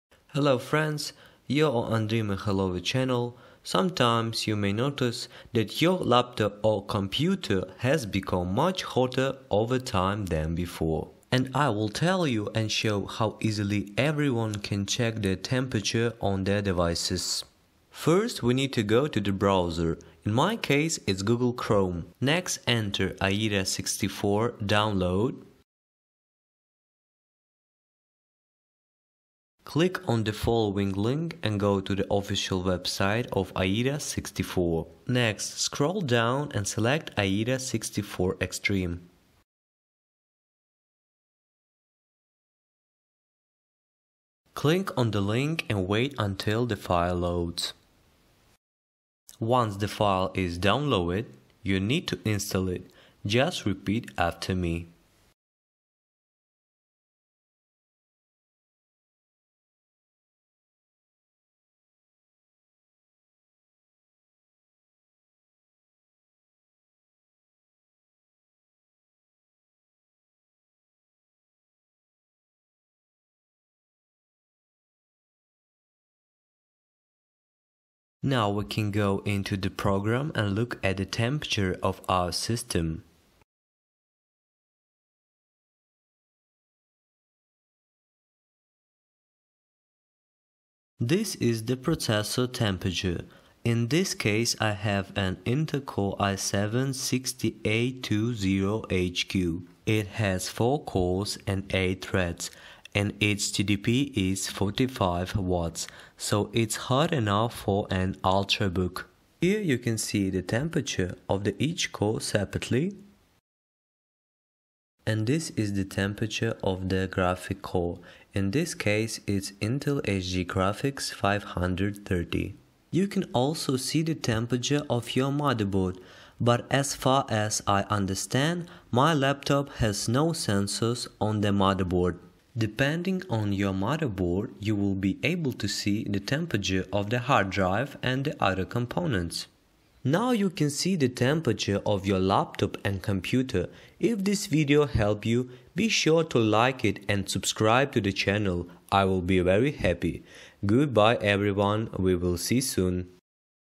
How to see cpu temperature windows 10 - AIDA64
Hello friends! In today's video, I will show how easily and fast you can find out the temperature of your processor and video card with the help of Aida64.